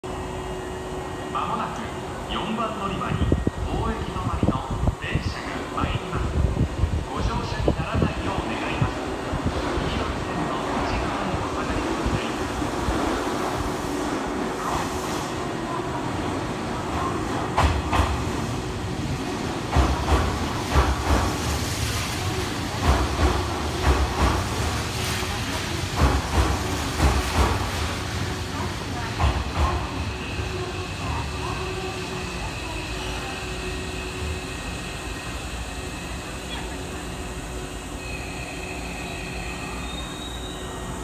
阪神電車 5700系 普通 終着の高速神戸駅に到着
阪神電車の5700系は，普通用車両では最も多い形式です。